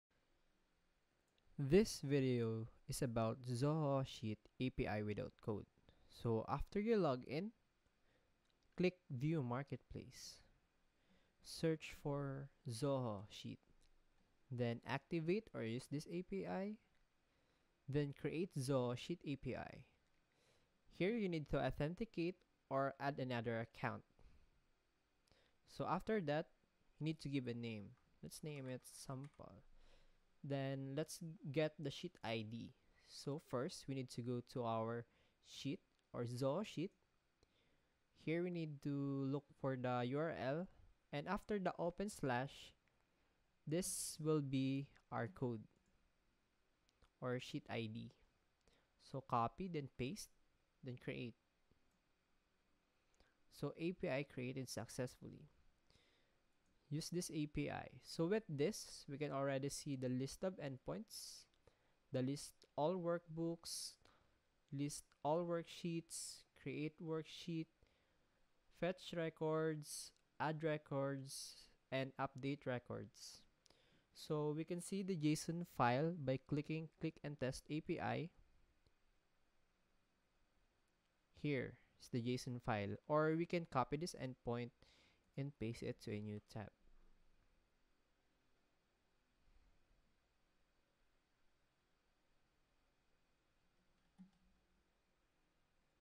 Zoho Sheet without code
Create a Zoho Sheet API configuration to access Workbook and Worksheet table data, add, update, and delete rows using a straightforward API without the need for server setup or scripting.